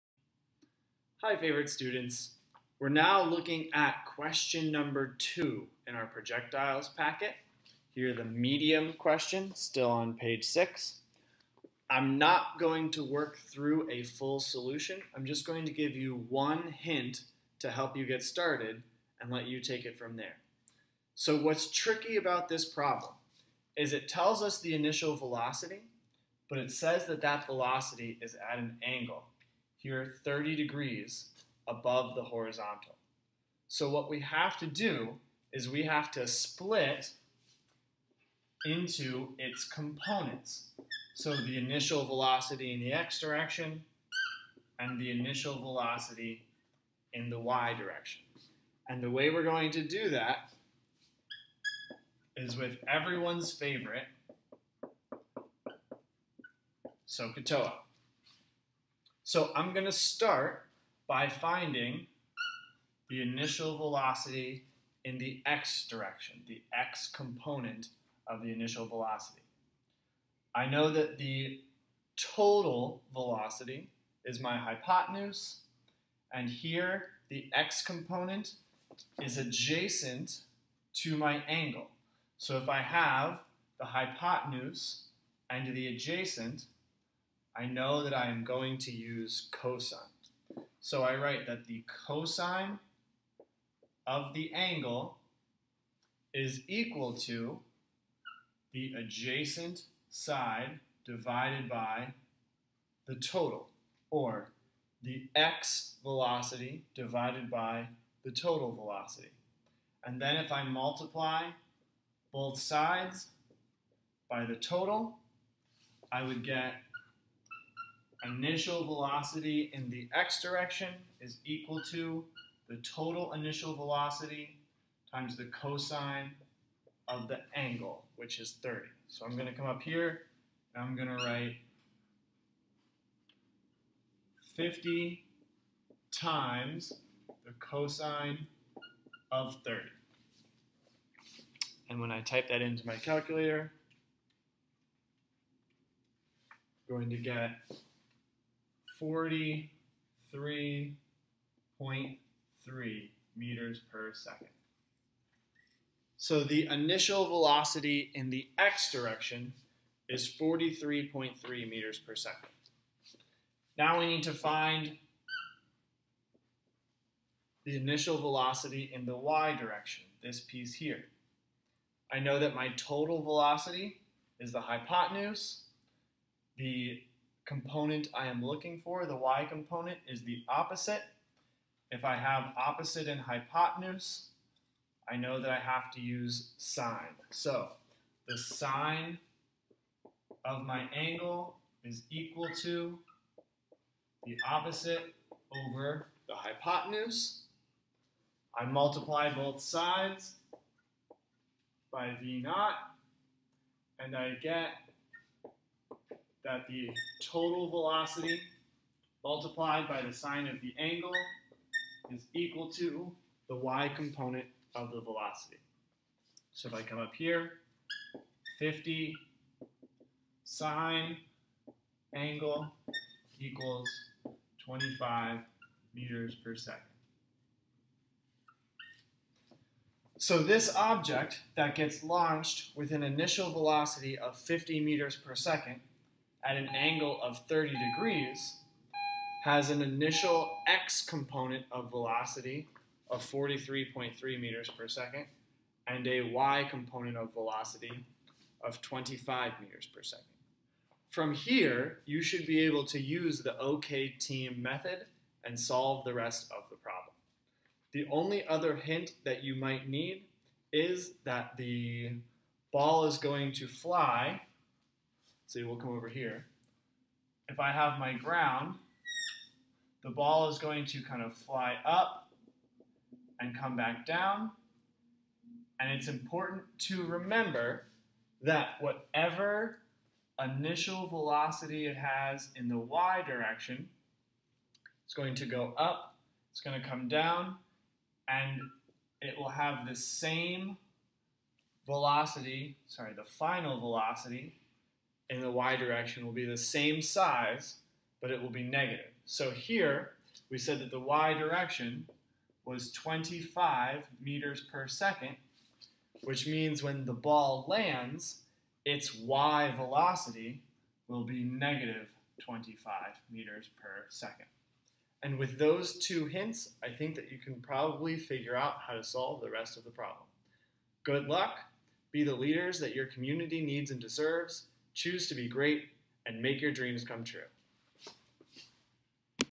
AP1 - Constant Acceleration Projectiles Problem 2
A hint (not a full solution) for projectiles problem #2 in your notes pack.